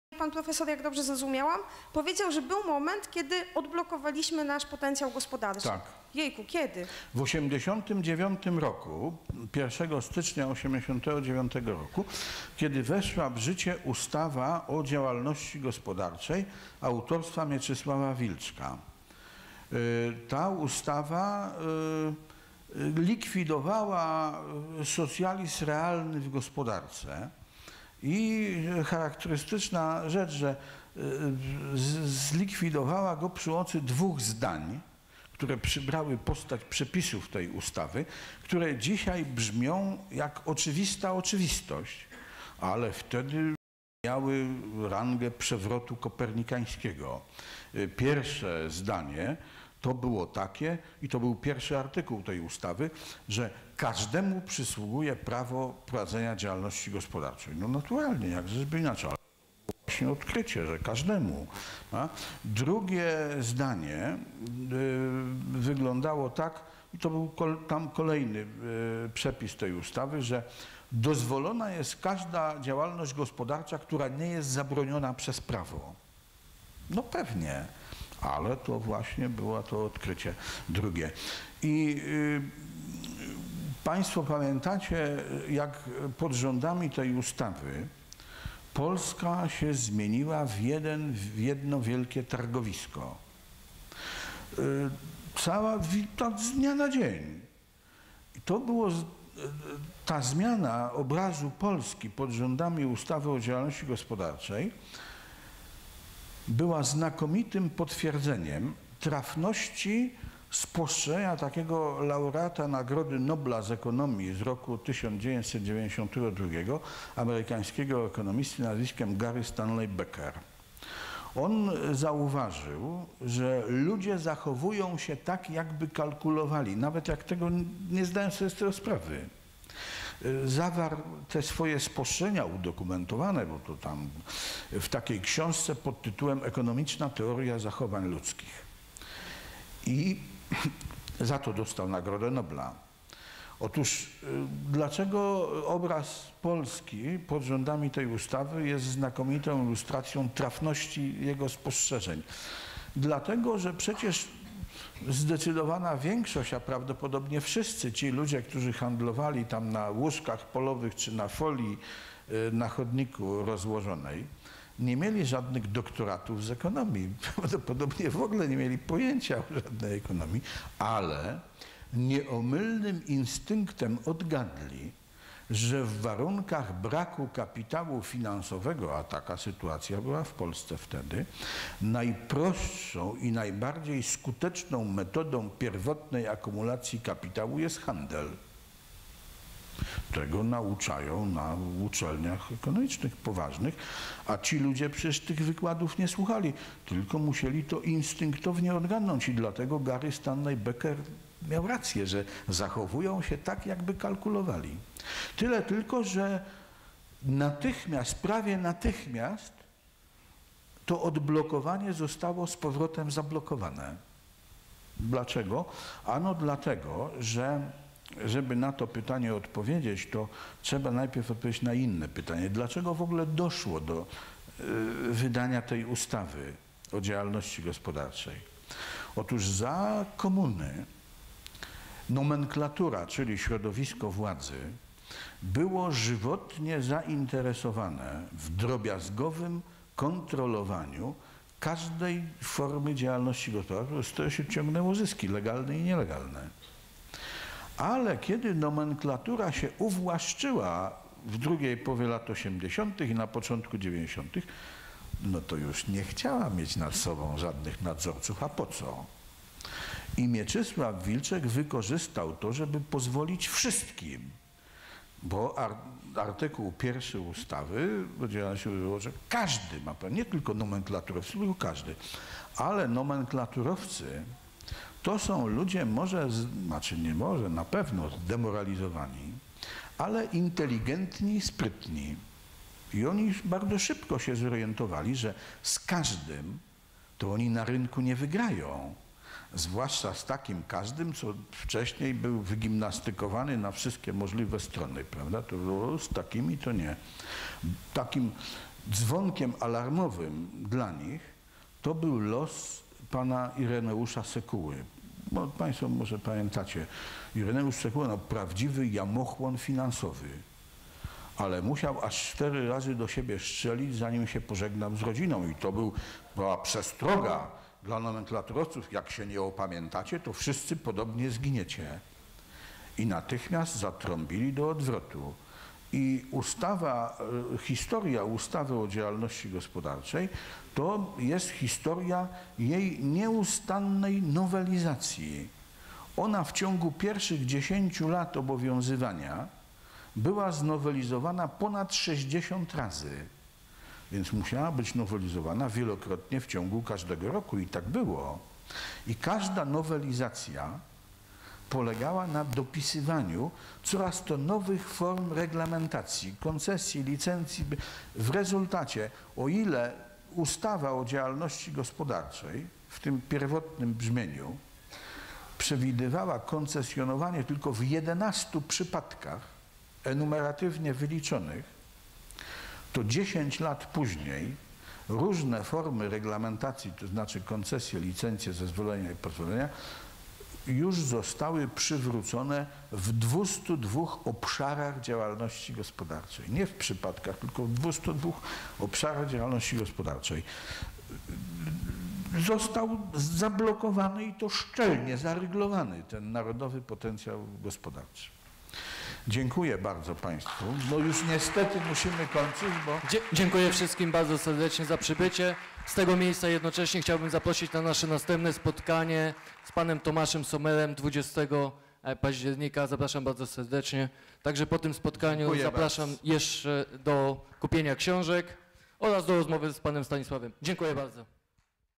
Stanisław Michalkiewicz o ustawie likwidującej socjalizm realny w gospodarce
Narodowy potencjał gospodarczy został zablokowany szczelnie.
Historia ustawy o działalności gospodarczej